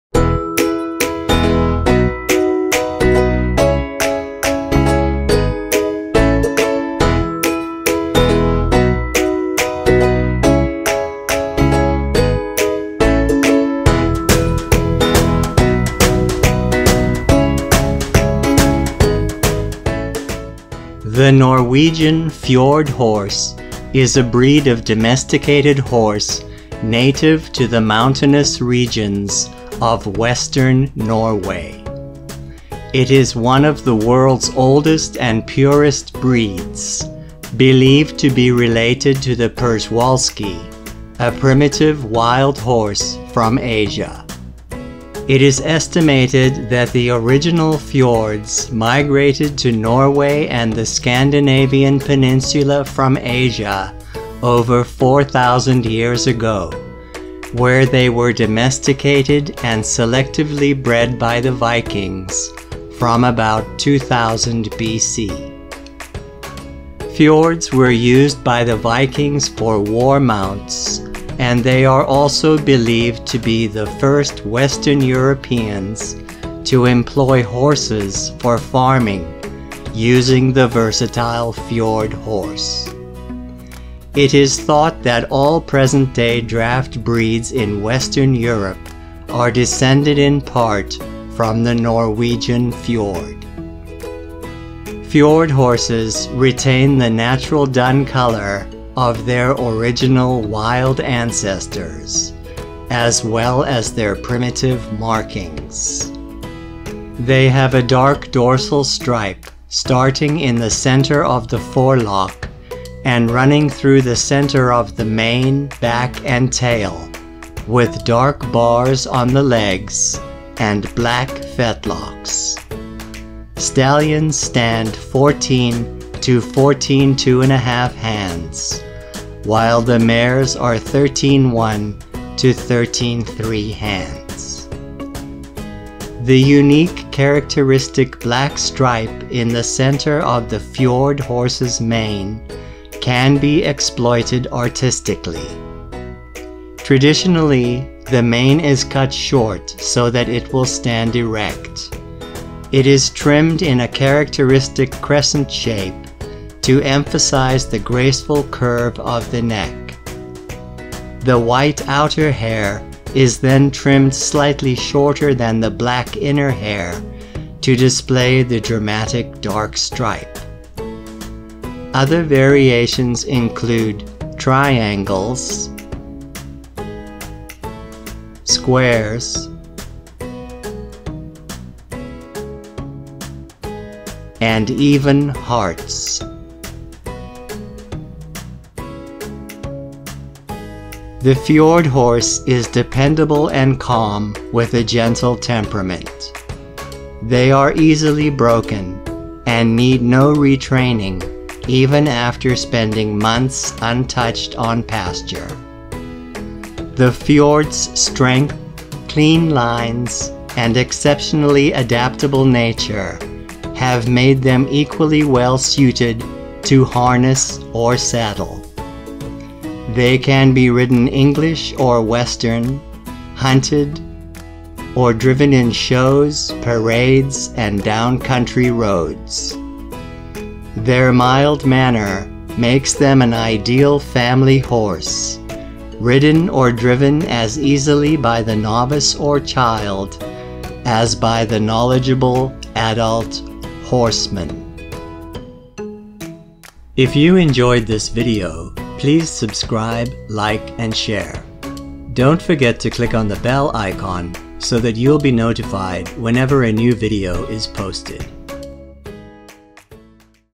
Norwegian Fjord Horses | Beautiful Thor(ed) Or Frozen
The Norwegian Fjord Horse is dependable and calm with a gentle temperament.